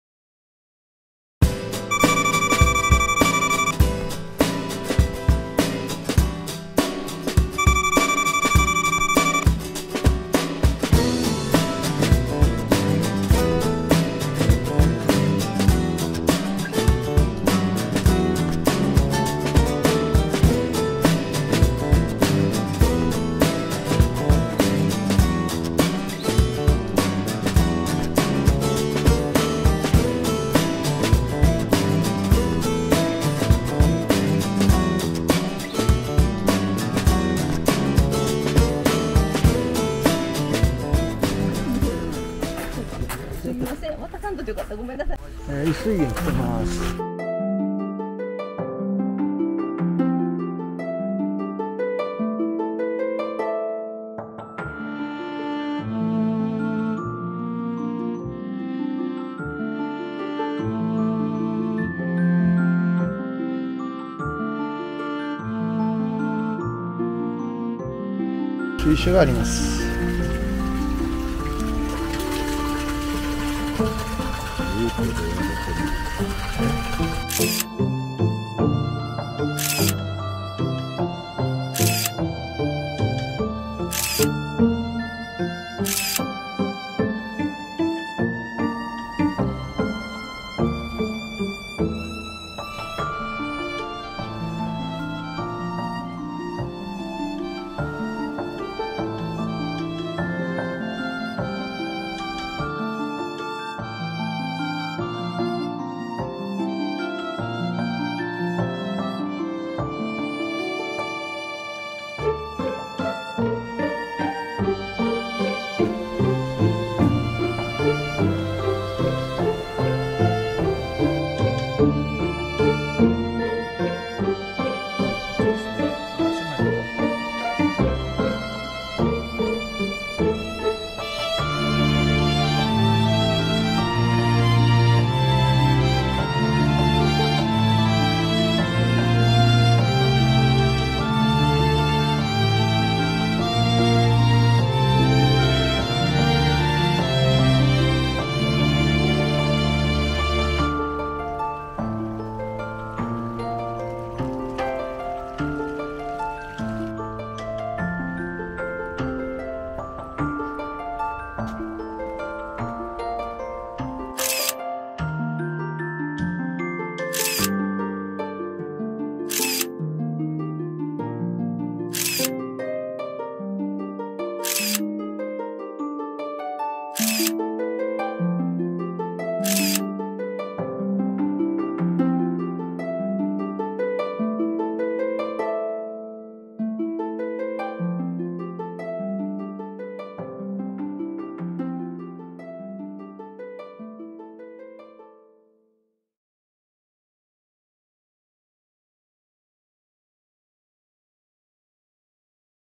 新緑の季節めぐり 依水園 奈良2024.5.20
桜の季節が終わりを告げたと思ったらもう5月も下旬に。
木々の若葉が芽生えて、美しい新緑の季節がやってきました。
平日にも関わらず、多くの観光客、特に外国人観光客が多く訪れていました。
定番カットを撮るのに順番待ちも!
庭園全体が「目に優しい」ライトグリーン一色で、自然とシャッターを押す回数が増えました。